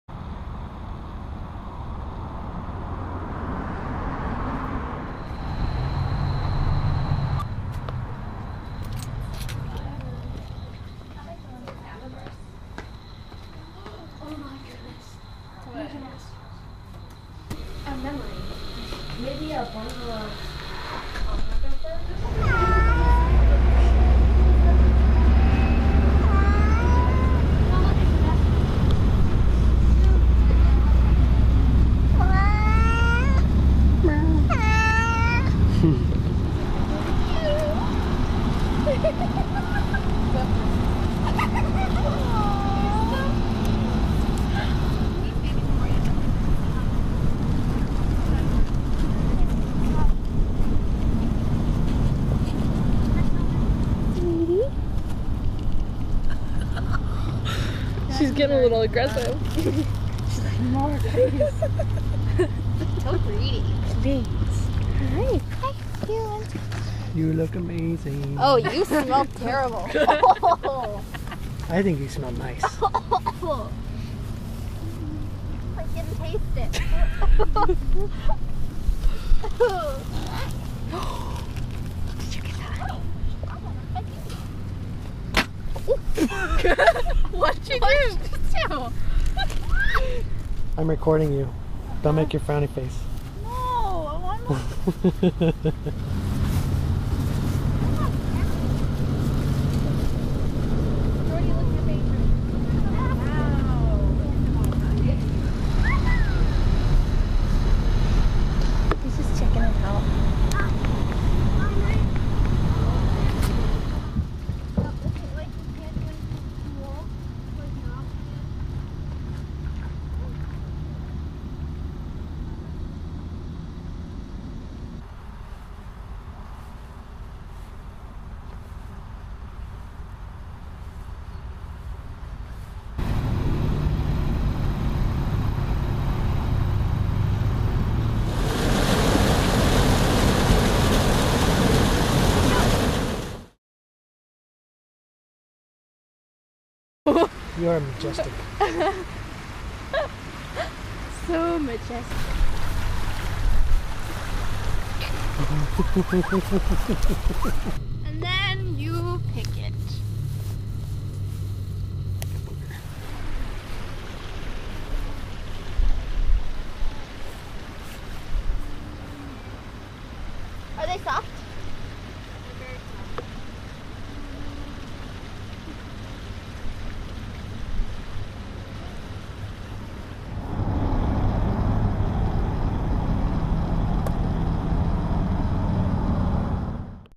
Full time RV Family: Visit to the Crescent Moon Alpaca Farm
Finally starting to sort through the video clips we shot while in central Oregon this September.  We really enjoyed the Crescent Moon Alpaca farm in Terrebonne, Oregon.   The video makes it look like we raced through, but we really spent about 2 hours here, feeding the alpacas, walking the grounds, and checking out the amazing (and amazingly expensive) things at the gift shop.  This is the first movie that I have edited, with Jeff helping me, so it's pretty basic, but I'm happy to finally be learning how to do it.